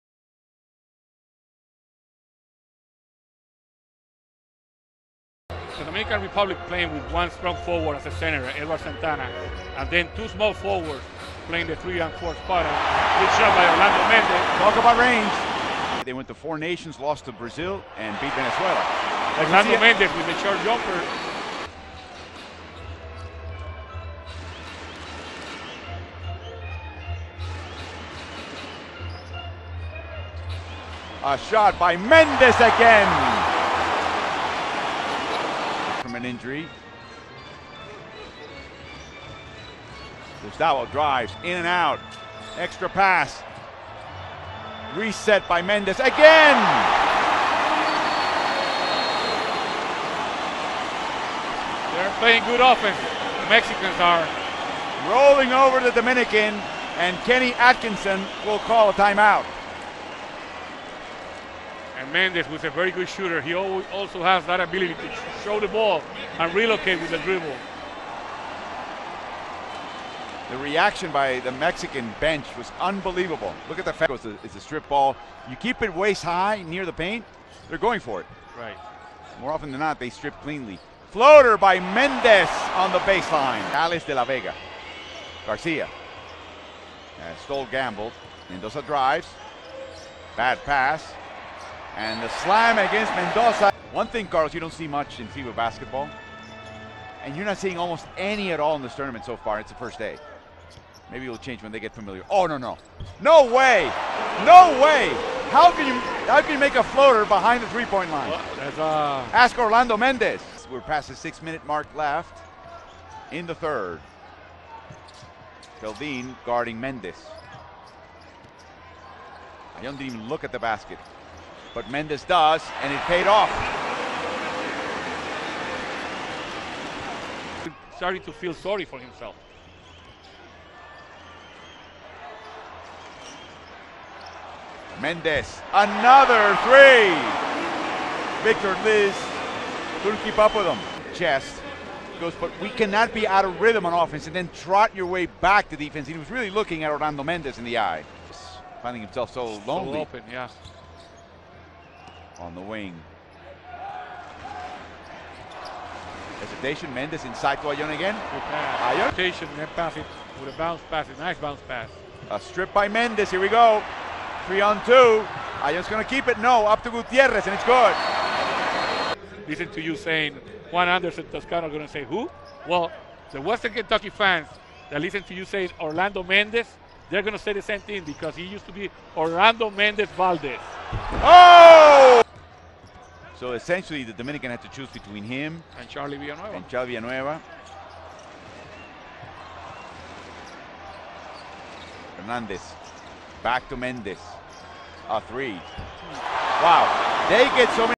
#13 Orlando Méndez-Valdéz Mex Vs Dom 31 agosto 2015
Preolímpico México 2015 (Palacio de los Deportes) CDMX
México 84 - 66 República Dominicana